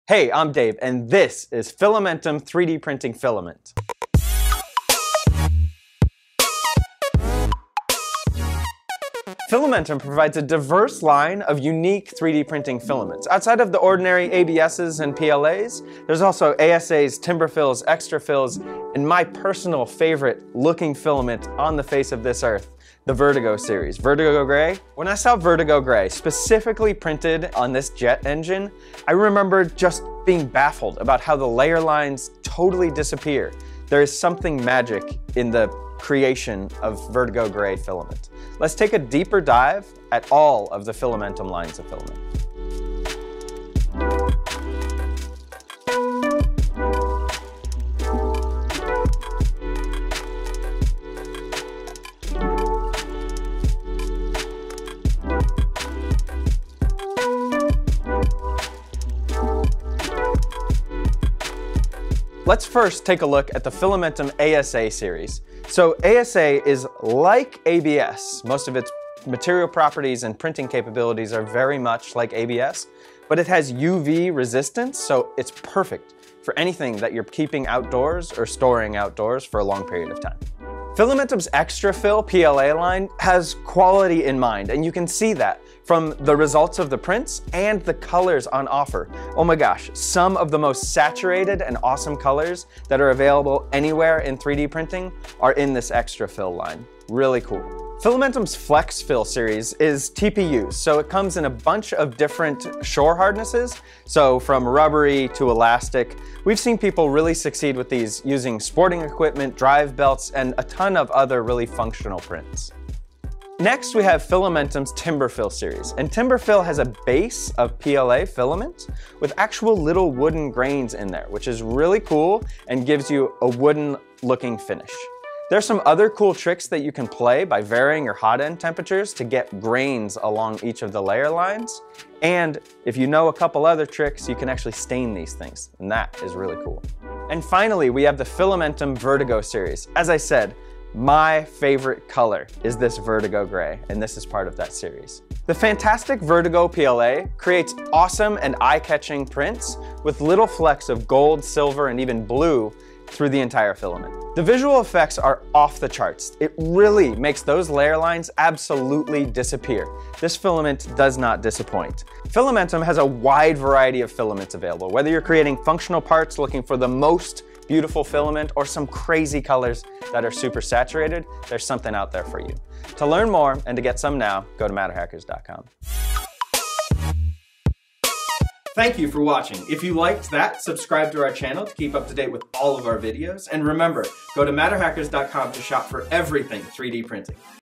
Fillamentum // 3D Printing Filament Spotlight
Join Dave as we take a look at the all of the amazing 3D printing materials available from Fillamentum. From ExtraFill, FlexFill,  TimberFill, and Crystal Clear, to their most popular series, Vertigo - your eyes will thank you! Check it out!